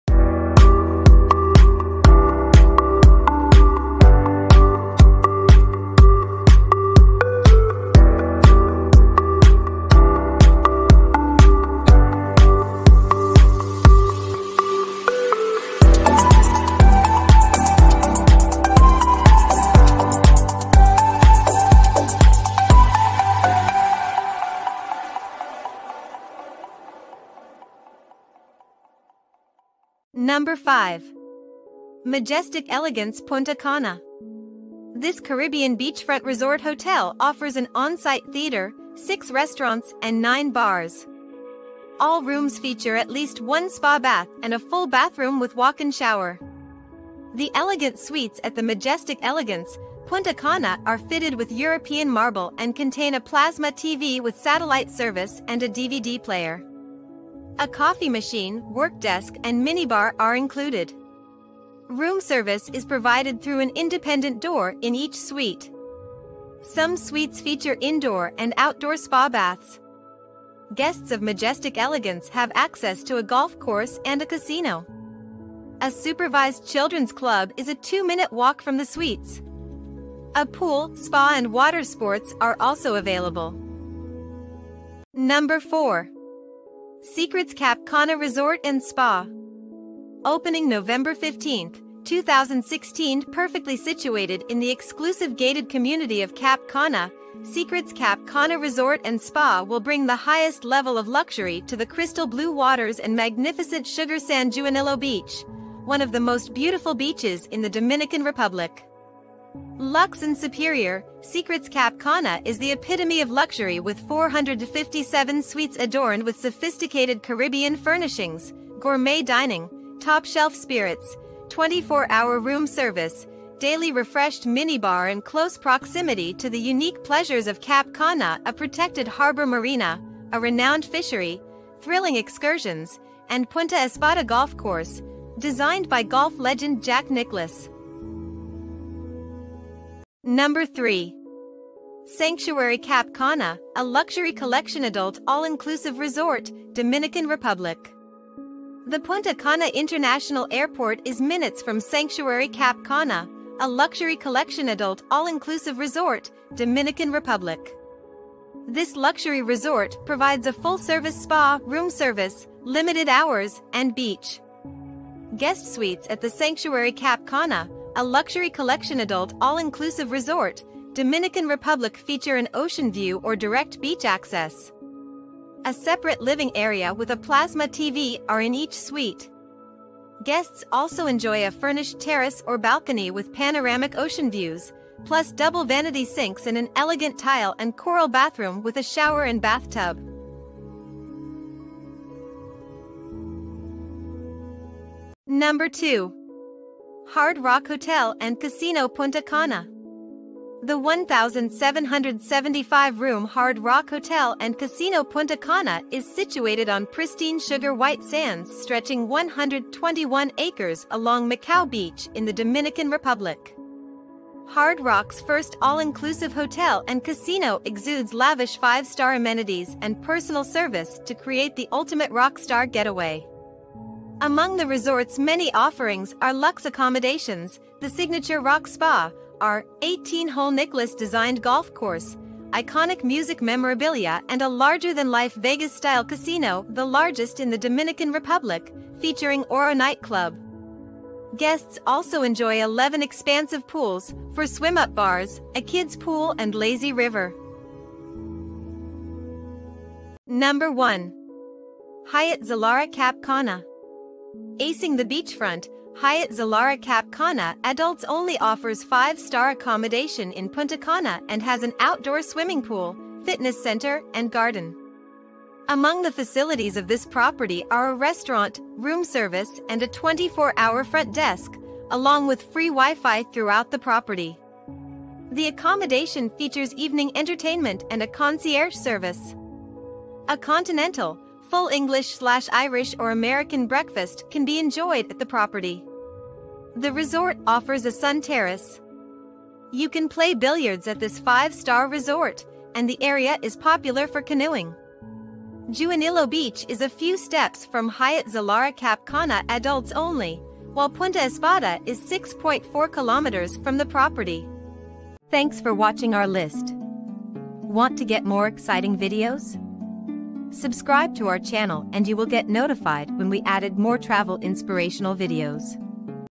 TOP 5 BEST all-inclusive luxury resorts in PUNTA CANA, Dominican Republic [2023, PRICES, REVIEWS]
In this video, we listed the top 5 all-inclusive luxury resorts in PUNTA CANA, Dominican Republic. We made this list based on personal opinion and tried to list them based on their price, quality, location, and more.

Check the prices and availability:
Top 5. Majestic Elegance Punta Cana
Top 4. Secrets Cap Cana Resort & Spa.
Top 3. Sanctuary Cap Cana
Top 2. Hard Rock Hotel & Casino Punta Cana
Top 1. Hyatt Zilara Cap Cana

Timeline
0:00 Intro
0:30 Top 5. Majestic Elegance Punta Cana
1:25 Top 4. Secrets Cap Cana Resort & Spa.
2:20 Top 3. Sanctuary Cap Cana
3:15 Top 2. Hard Rock Hotel & Casino Punta Cana
4:10 Top 1. Hyatt Zilara Cap Cana